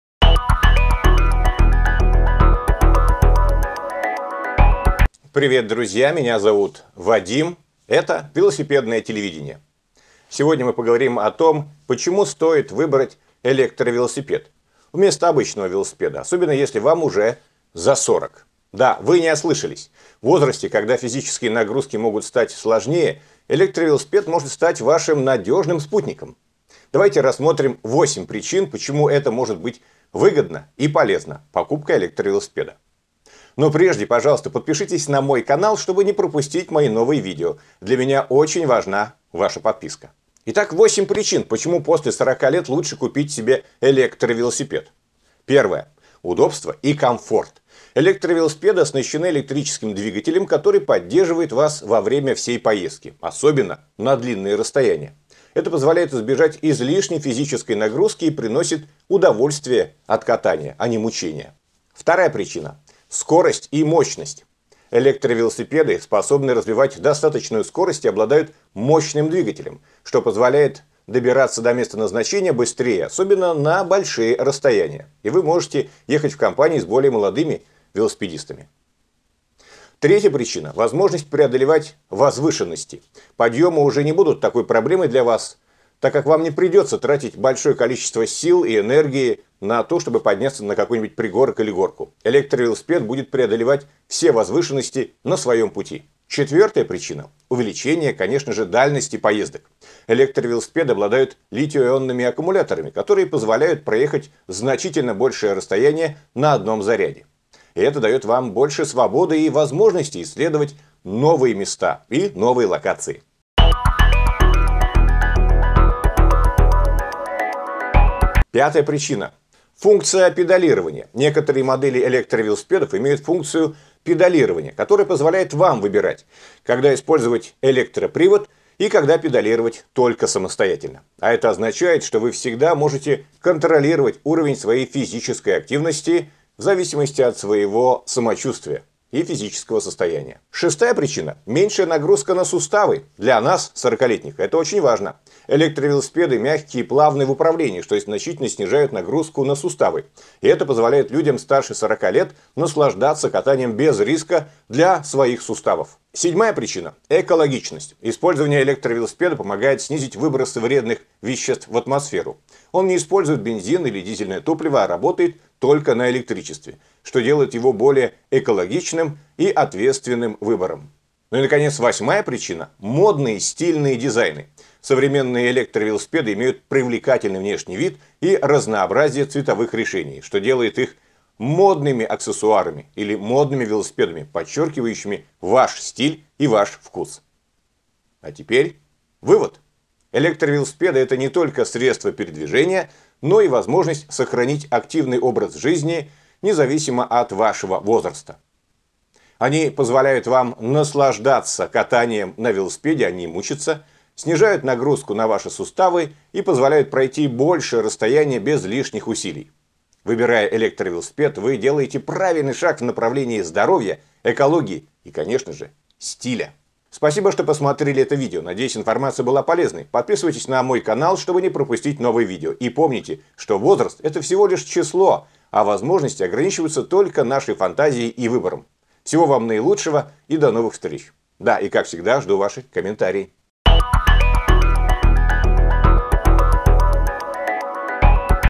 8 ПРИЧИН, ПОЧЕМУ ЛЮДЯМ ЗА 40 ЛЕТ НУЖЕН ЭЛЕКТРОВЕЛОСИПЕД! НЕ ПОКУПАЙТЕ ПРОСТОЙ ВЕЛОСИПЕД!
8 ПРИЧИН, ПОЧЕМУ ЛЮДЯМ ЗА 40 ЛЕТ НУЖЕН ЭЛЕКТРОВЕЛОСИПЕД! НЕ ПОКУПАЙТЕ ПРОСТОЙ ВЕЛОСИПЕД! В этом видео я расскажу вам о 8 преимуществах электровелосипеда для людей за 40 лет. Вы узнаете, почему электровелосипед - это не просто средство передвижения, а ключ к активной, здоровой и счастливой жизни.
Смотрите видео и узнайте, как электровелосипед может изменить вашу ЖИЗНЬ!